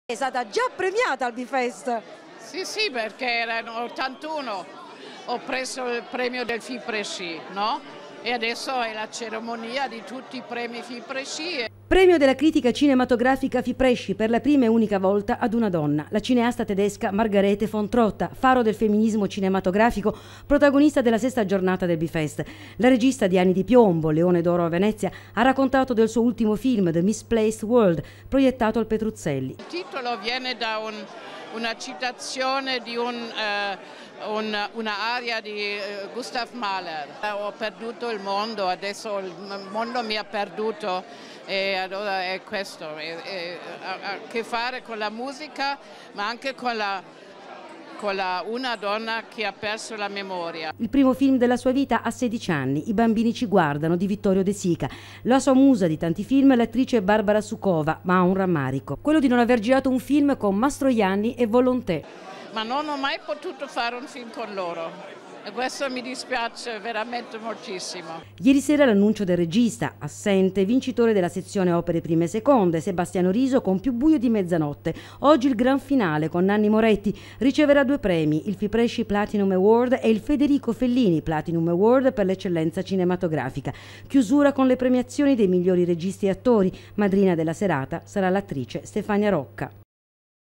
BARI BIFEST CON MARGARETHE VON TROTTA
ALLE BATTUTE FINALI IL BIFEST A BARI. IERI PROTAGONISTA DELLA RASSEGNA LA GRANDE REGISTA TEDESCA MARGARETHE VON TROTTA. GRAN FINALE CON NANNI MORETTI